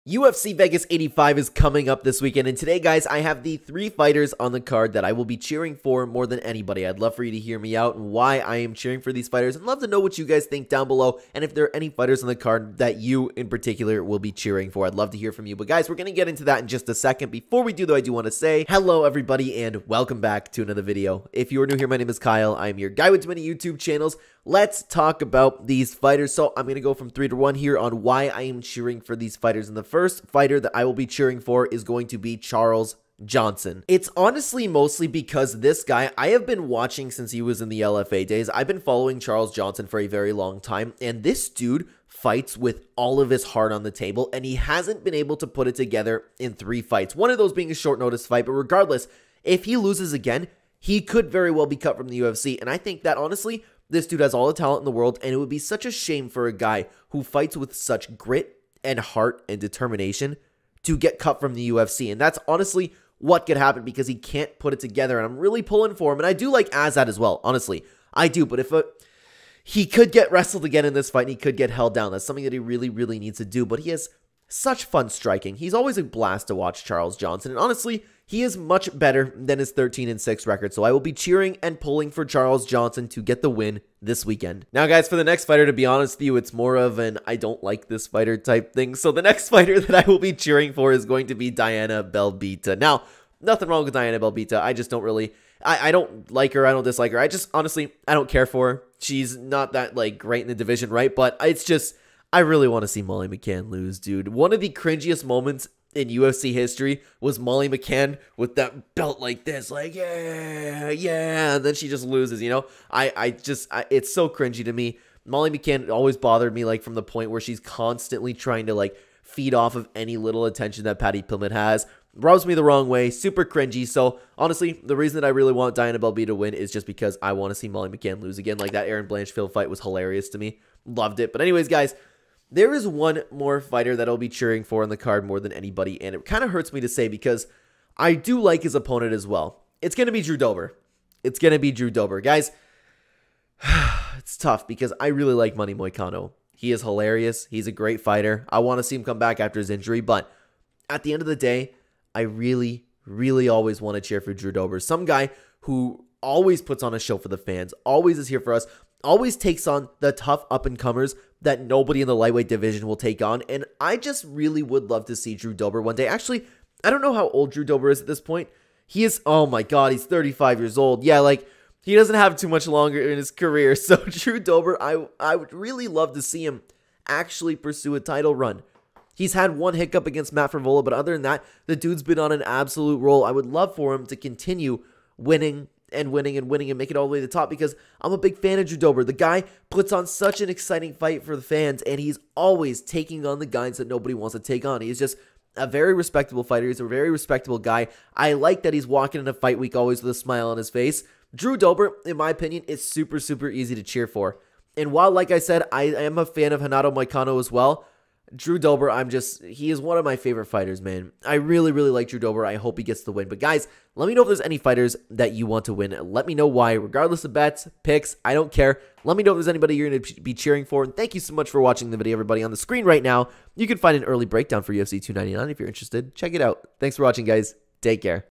Top 3 Fighters I Want to Win at UFC Vegas 85 - Here's Why
Out of the full card for the next UFC Fight Night, here is who I will be cheering for!

Connecting & Socials:

3. Discord Invite (Help Make Videos)